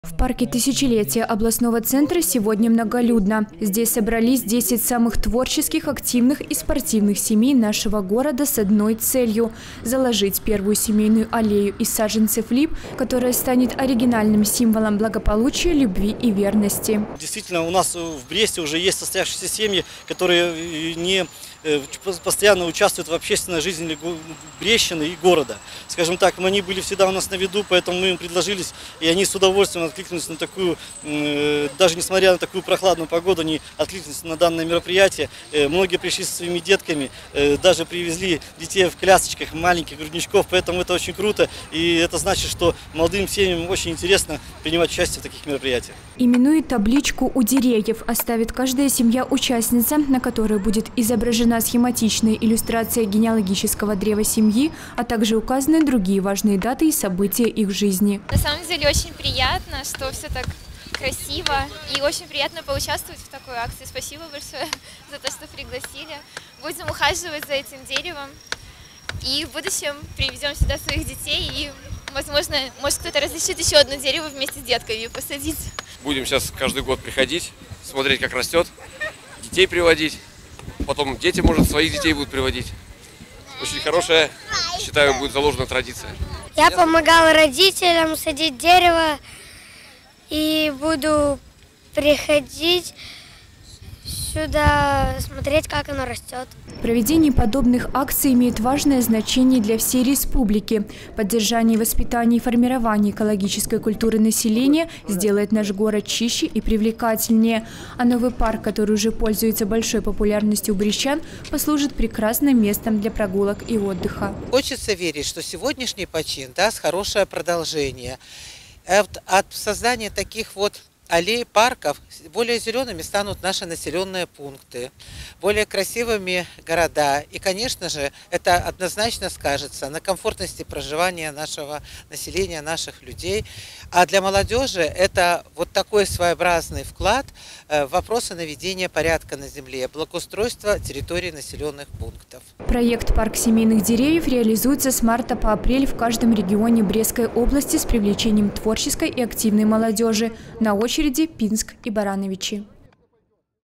2019-03-15 г. Брест. Старт респ. проекта «Парк семейных деревьев». Новости на Буг-ТВ.
Журналист Ю. Силюк. Оператор А. Щерба
В городе над Бугом дан старт республиканскому проекту «Парк семейных деревьев». Эта инициатива была предложена главе государства еще осенью прошлого года ОО «БРСМ». В Бресте  основными организаторами проекта выступили   областной комитет ОО «БРСМ» и   областной комитет природных ресурсов и охраны окружающей среды, при поддержке городского исполнительного комитета.